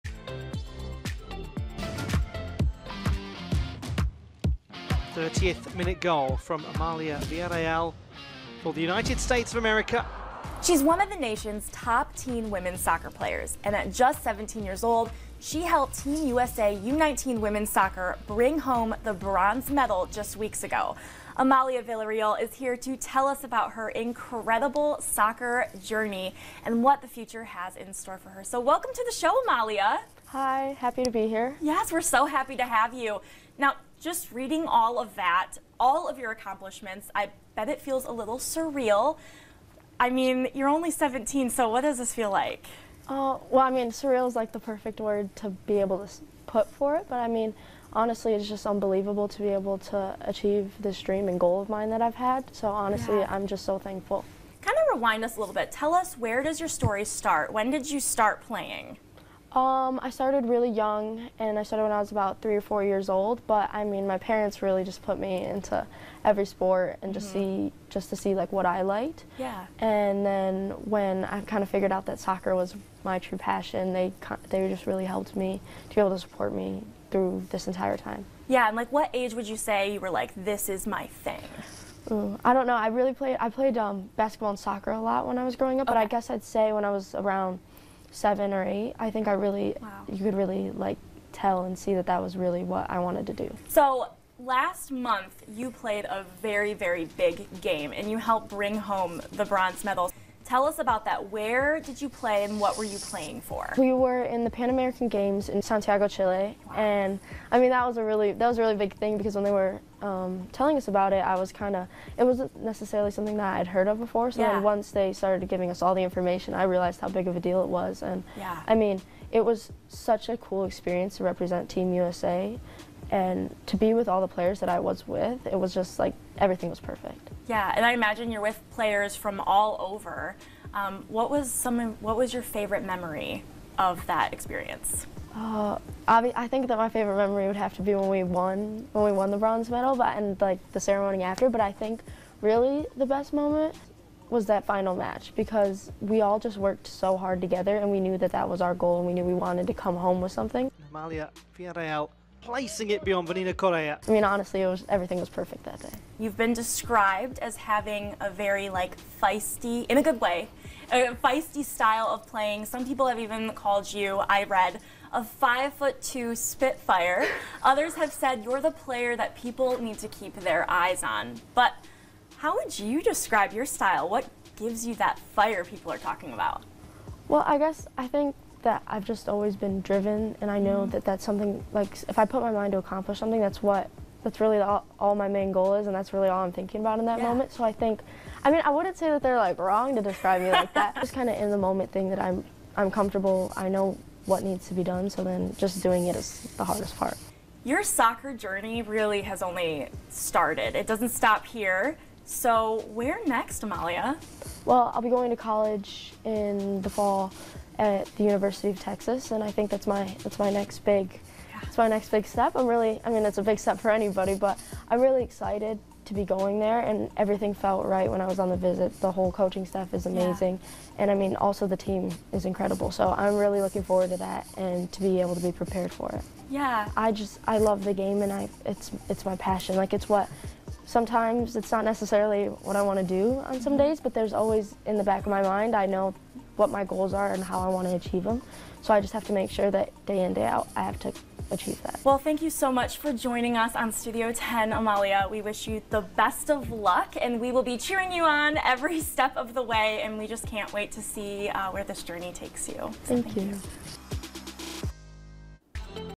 Kickin' It with Team USA's #9
At just 17, Amalia Villarreal has already reached a lot of her goals... literally. Learn about the Lansing native's experience playing for the USA U019 Women's National Team at the 2023 Pan-American Games in Santiago Chile, and how she helped them bring home the bronze!